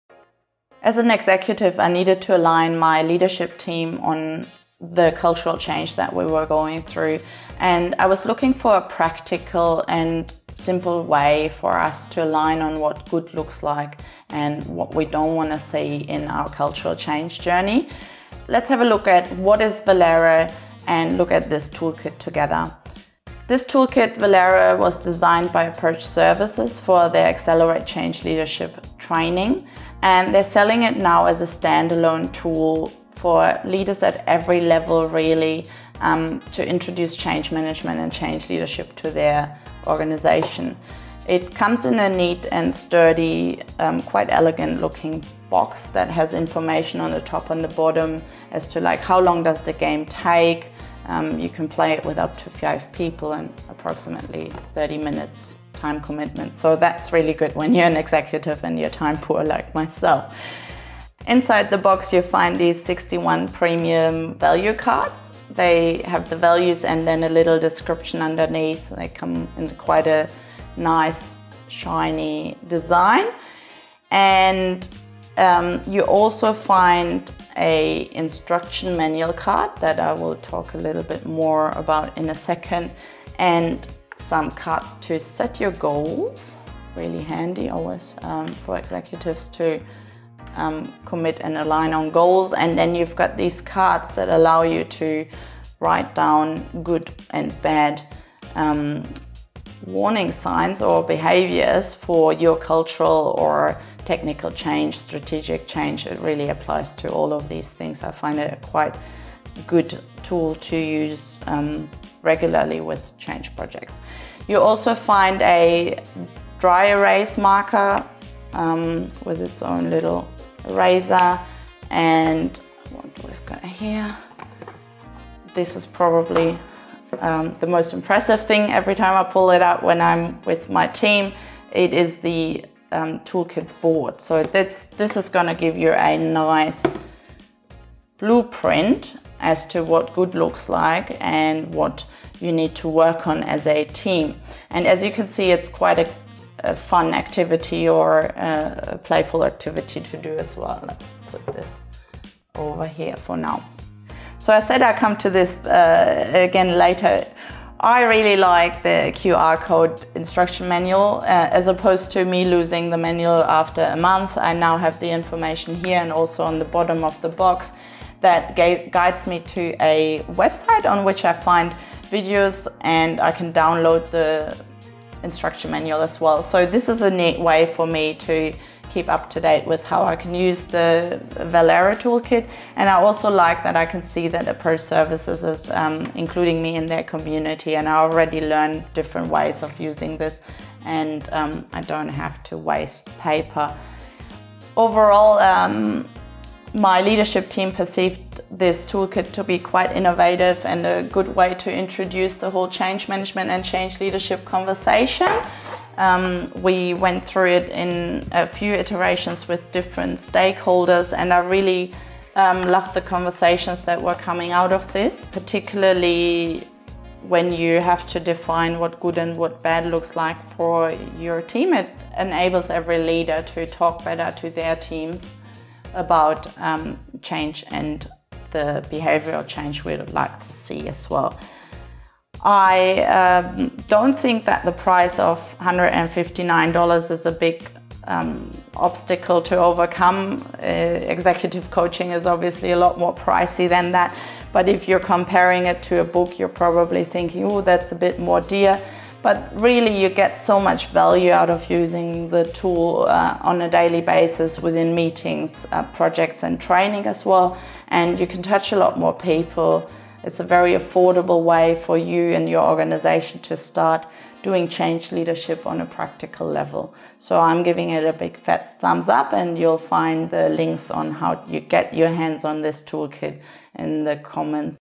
Why I love the Valēre Value Cards - And you should too!
I found the VALĒRE toolkit when I was looking for an evidence-based and practical way to align my leadership team for this cultural change in our business. In this video, I review the toolkit, share my highlights and its value for money.

Valēre allows you to:

✔ Reflect on core and important values
✔ Define what good looks like
✔ State behaviour that will be called out

Happy change!